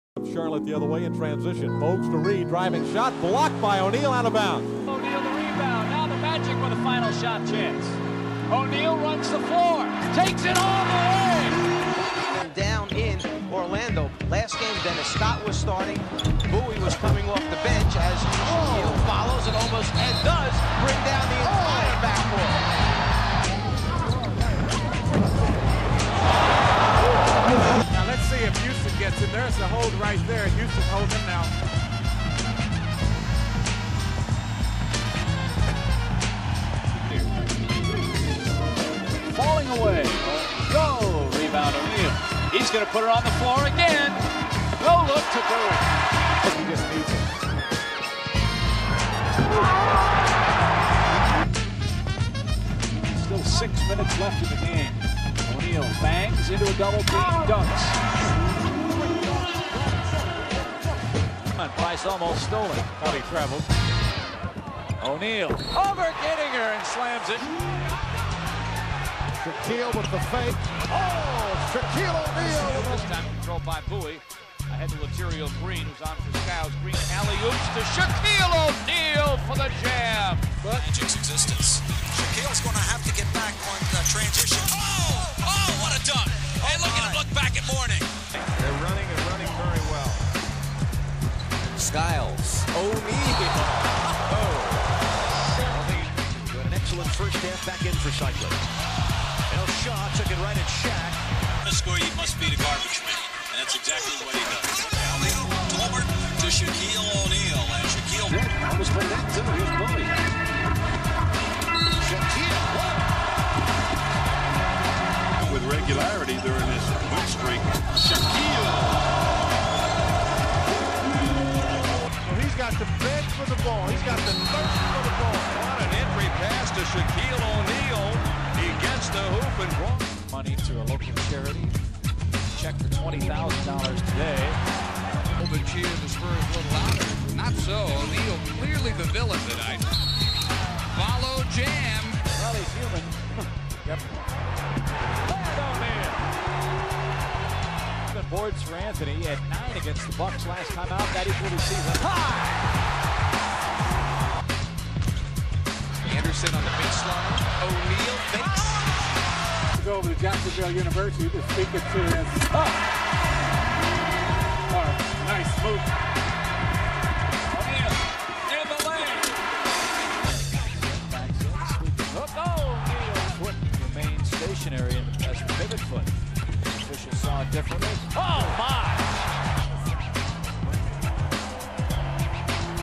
Shaquille O'Neal UNREAL Rookie Season Highlights
Shaquille O'neal Orlando Magic NBA Rookie Year Highlights 1993.